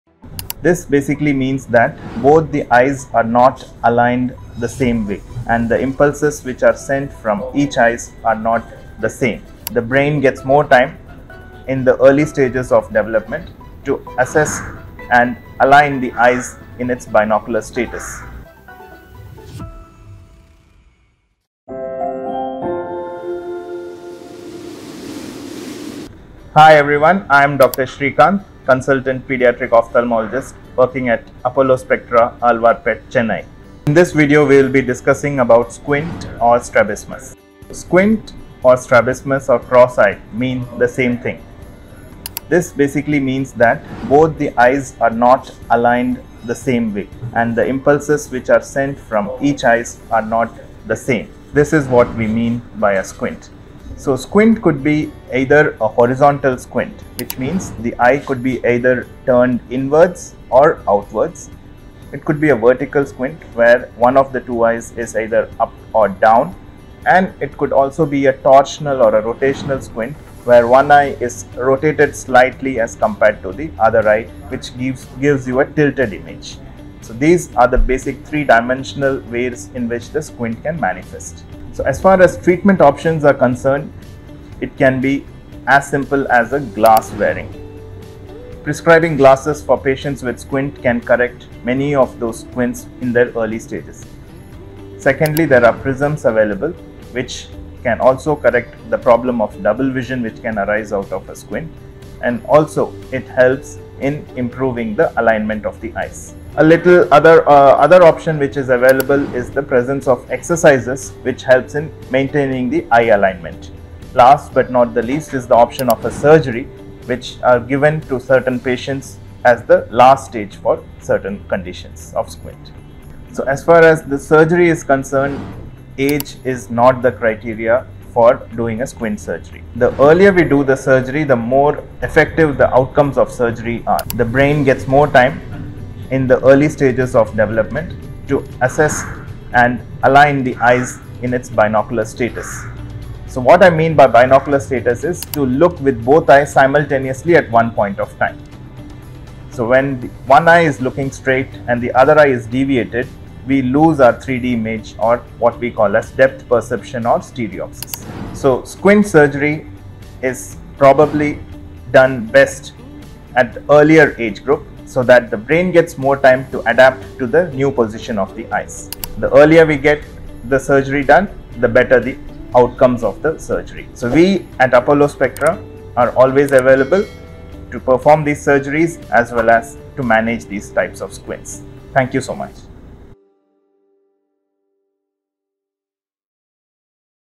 What causes Squint eye?
Squint eye is a phenomenon where both eyes are not aligned with each other. Watch Dr. Srikanth, Ophthalmologist from Apollo Spectra Hospitals talk about the causes, diagnosis & treatments related to Squint Eye.

Annotations:
0:38 – What exactly is squint or non-Alignment of eyes?
0:56 – What are the types of squints?
1:23 – What are the treatment options for Squint eye?
1:56 – What is a squint eye surgery?
2:44 – What is the best time to perform squint surgery?